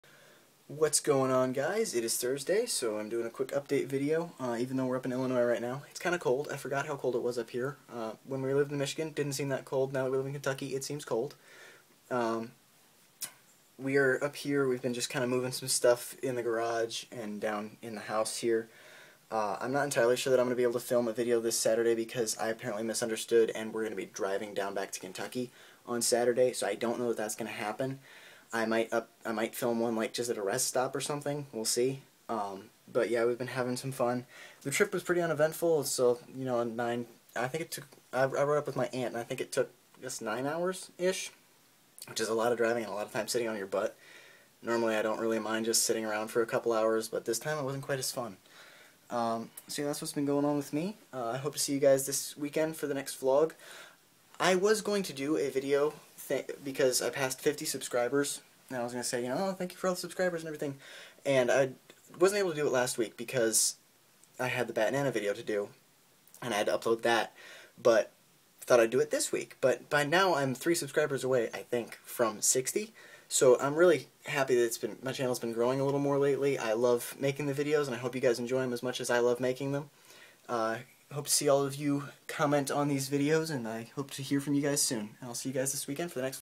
I'm Cold!
So, I'm in Illinois doing stuff.  It's a lot colder here than I remembered.  I'm not totally sure I'll have a video this weekend but let's hope it all works out!

Thank you so much for helping me reach (and pass) 50 subscribers!